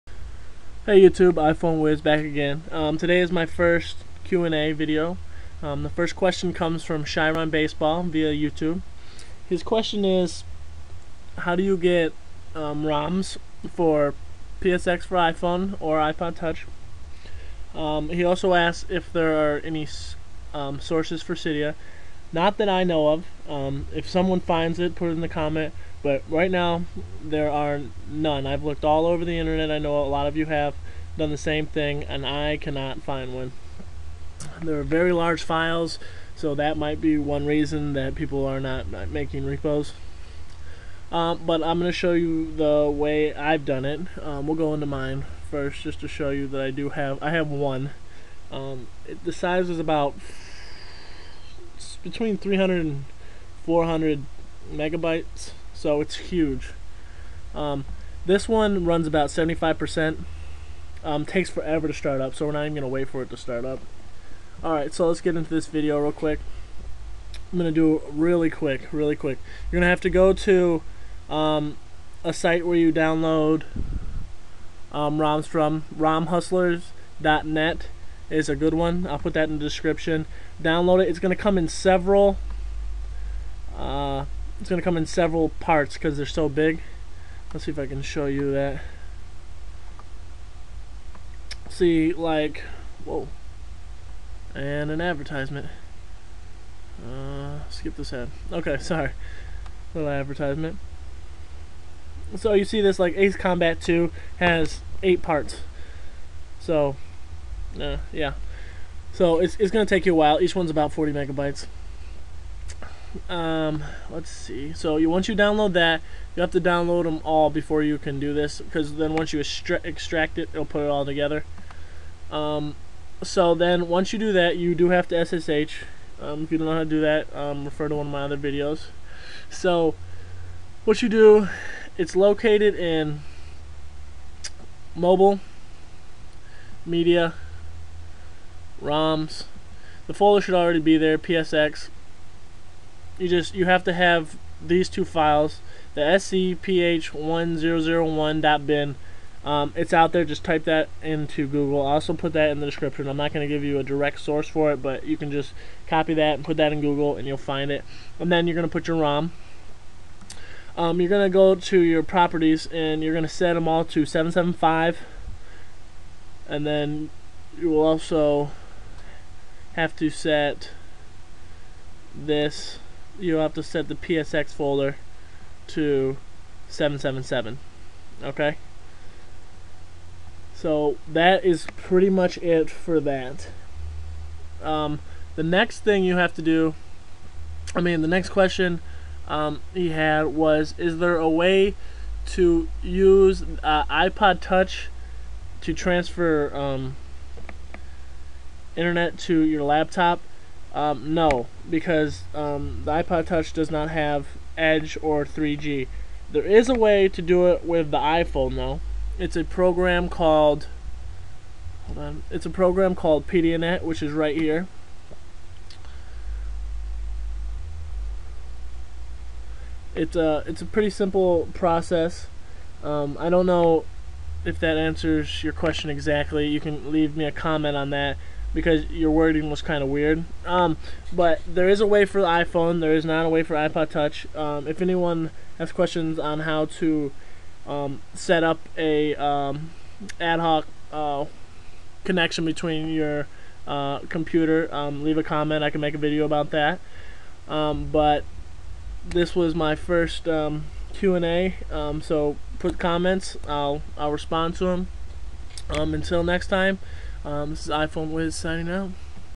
Q&A: How to add PSX roms and How to use your Iphone as a wireless router.
PSX needs:
scph1001.bin